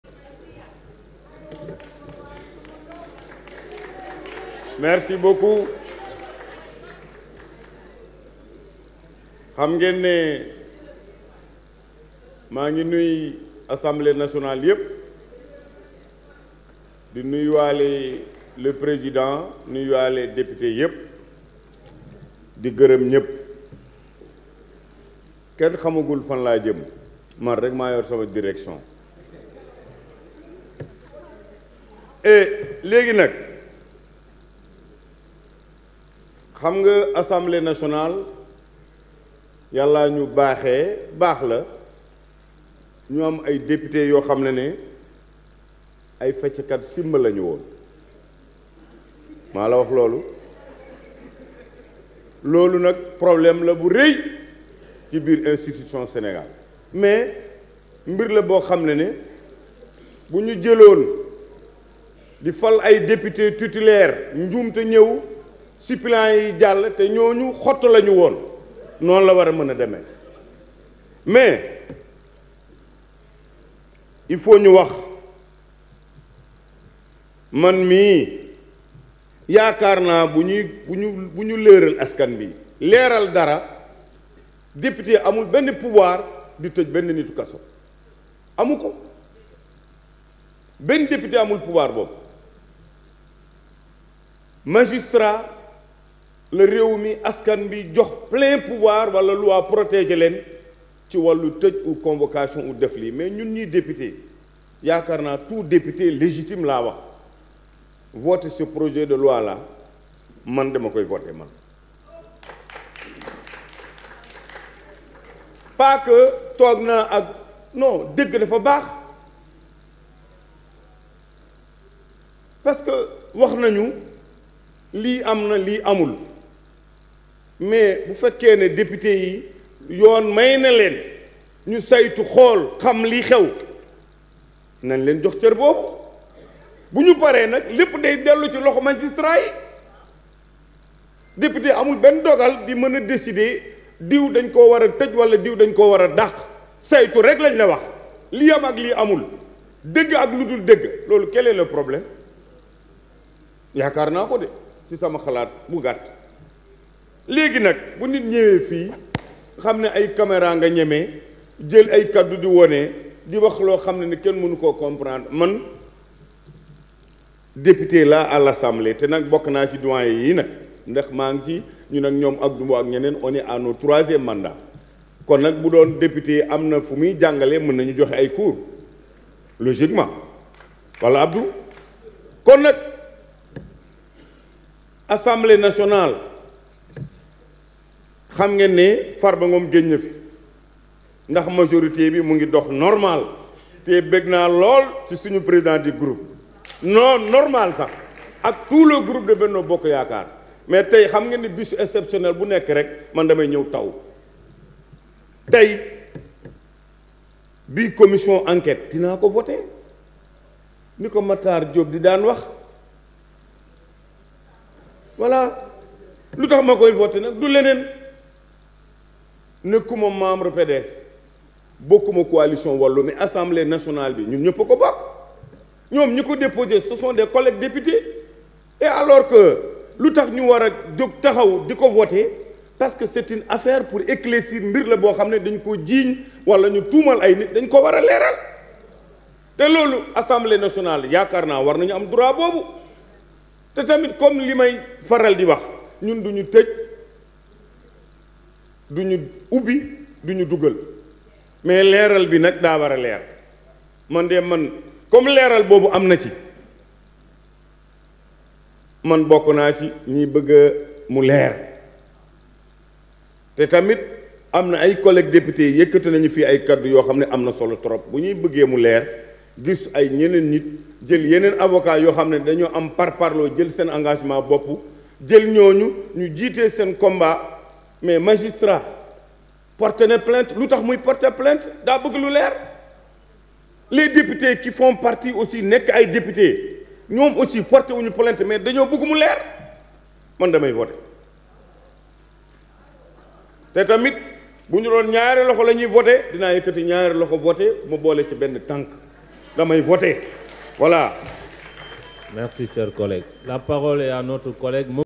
"Farba Ngom Affirme son Soutien à la Commission d'Enquête Parlementaire"
Direct Assemblée nationale : Comission d'enquête parlementaire